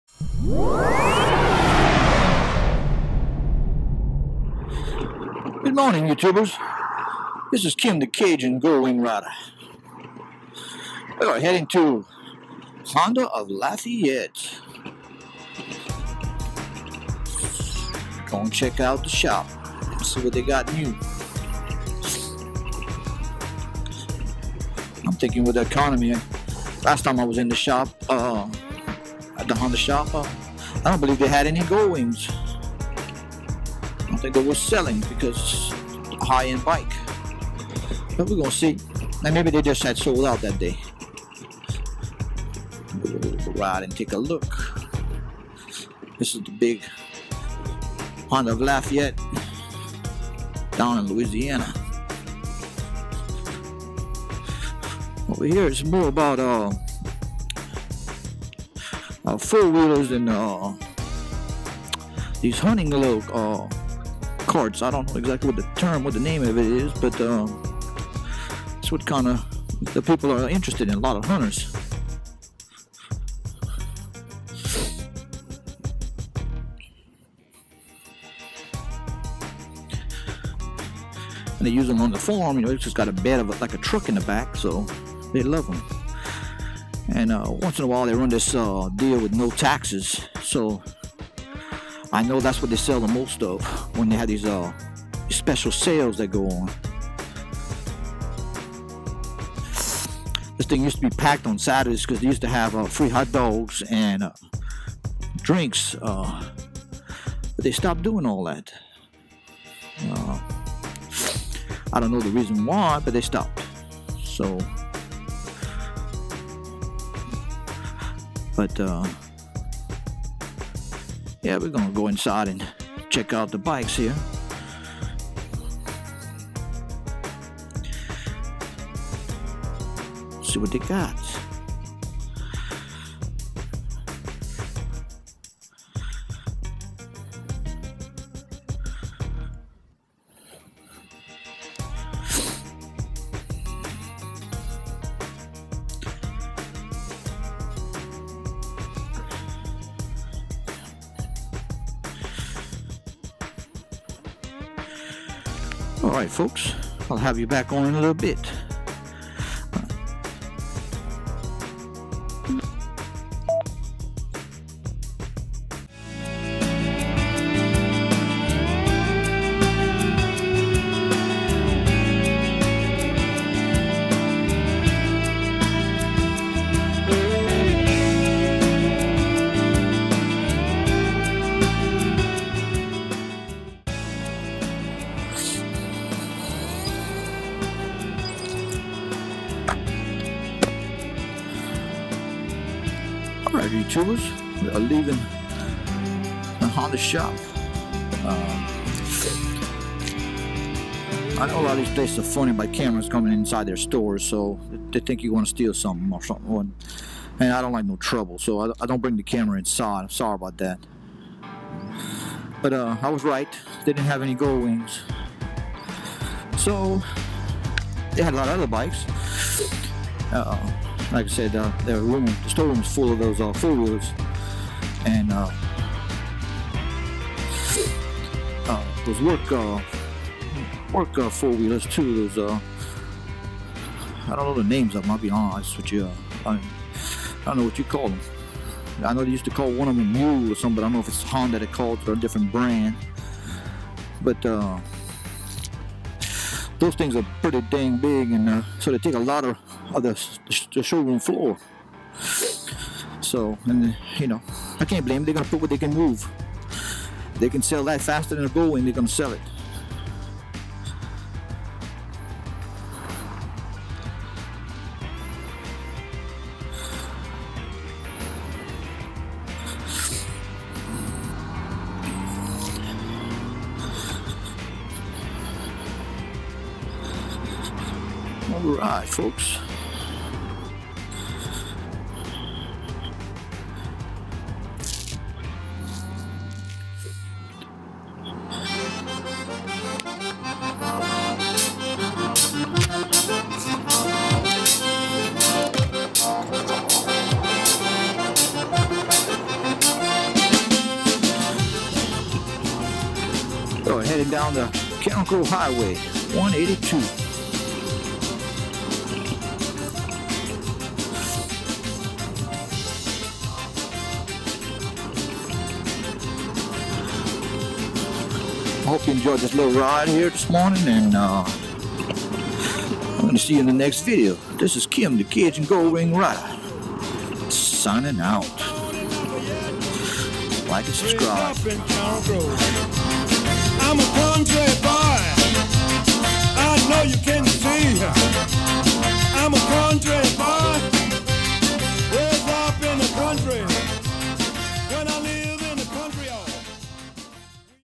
#20. Kim The Cajun Goldwing Rider (Cajun Goldwing #20)
Taking a Trip To Honda of Lafayette to check out The Goldwing . So Come for the ride .